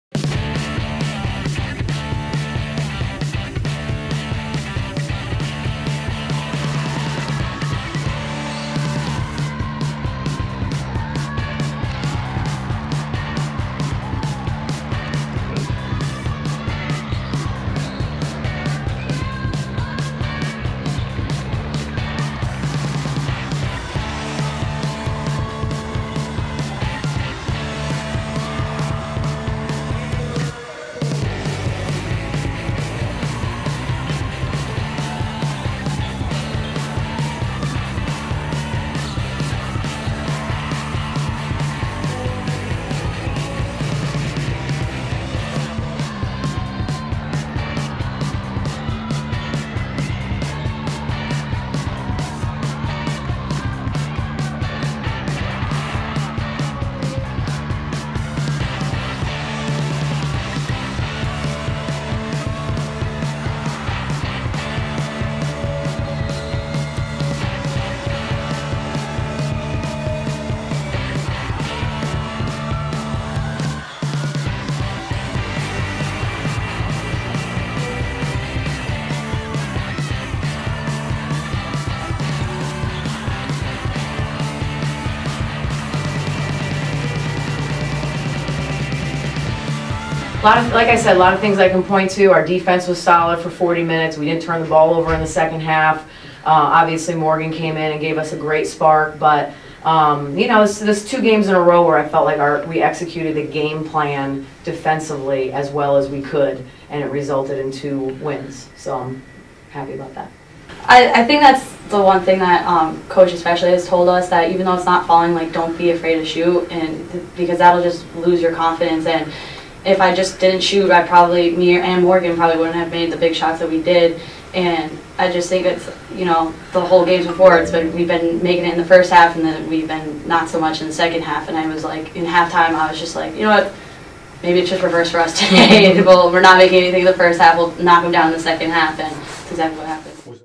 Women's Basketball vs Stony Brook 2.2.14
The Hartford women's basketball team used clutch shooting from Amber Bepko and Morgan Lumb as well as stellar free throw shooting to overcome an eight-point deficit in the second half to be the Stony Brook Seawolves, 61-48.